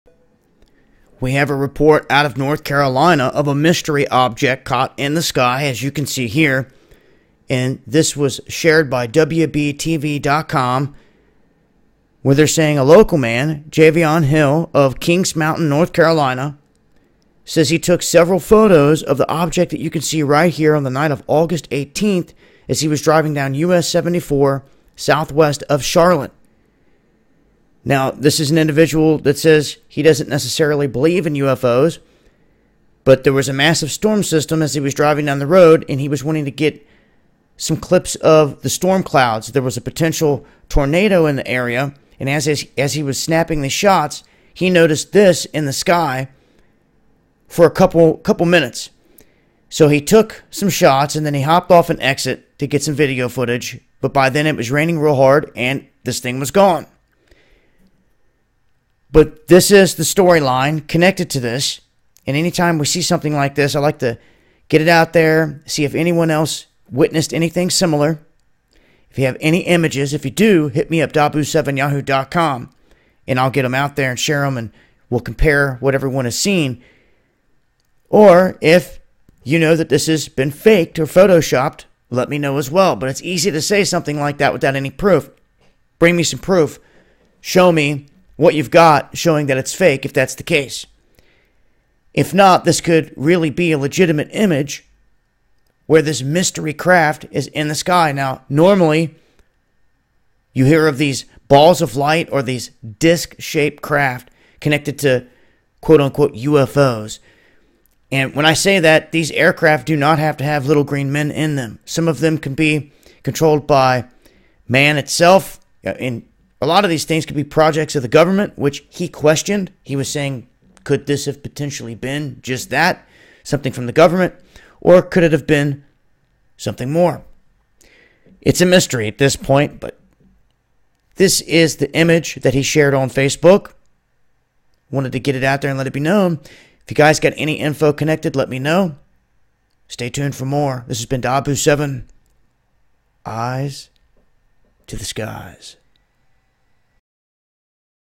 Mysterious Square Aircraft Seen In North Carolina
Another alleged UFO sighting near Charlotte has appeared on Facebook and in this case, it was posted by a husband and father who says he doesn’t necessarily believe in UFOs.

Javion Hill, 35, of Kings Mountain, North Carolina, says he took several photos of the object during a storm on the night of Aug. 18, as he drove on U.S. 74 southwest of Charlotte.

The images feature something square hovering above the treeline, with its edges fringed in lights. Hill told the Charlotte Observer the craft frightened him to the point that he didn’t sleep that night.

“I was on my way home with my wife on the speakerphone, and it looked like there was a tornado coming, so I was trying to take a picture of the clouds for my wife,” says Hill.

Bitcoin- 1Nmcbook8TwAdtZHsMdVxRtjBnyrSArDH5